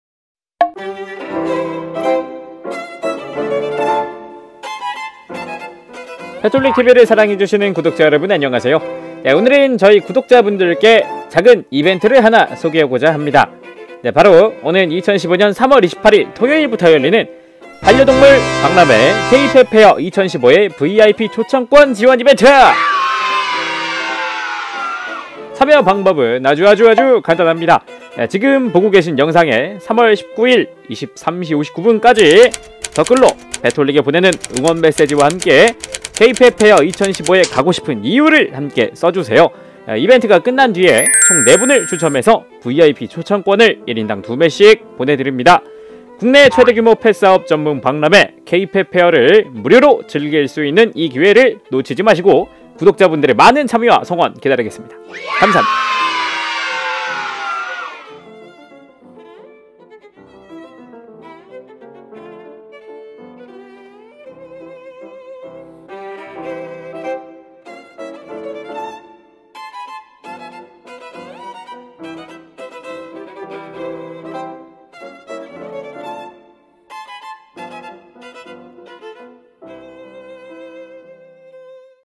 [첫 이벤트 : 마감] 2015 K-PET FAIR (케이펫 페어) VIP 초청권 지원 이벤트
펫 홀릭 - [첫 이벤트] 2015 K-PET FAIR (케이펫 페어) VIP 초청권 지원 이벤트

1. 참여 기간 : 3/13(금) ~ 3/19 (목)
2. 참여 대상 : 펫 홀릭 TV 구독자이면 누구나 참여 가능
3. 참여 방법 : 본 영상에 댓글로 펫 홀릭 TV 응원 메시지와 가고 싶은 이유를 남겨 주시면 됩니다.. ^^

그동안 베풀어 주신 구독자 여러분들의 따뜻한 성원에 작게나마 보답드리고자 펫 홀릭 TV에서 준비한 "작은 이벤트" 입니다.

앞으로도 다양한 이벤트를 만들어 구독자 여러분의 관심과 사랑에 보답하고자 최선의 노력을 다하겠습니다.

펫 홀릭 TV에서 준비한 첫 이벤트에 여러분의 많은 홍보와 참여 부탁 드립니다.
사랑합니다. 그리고 감사합니다.. ^^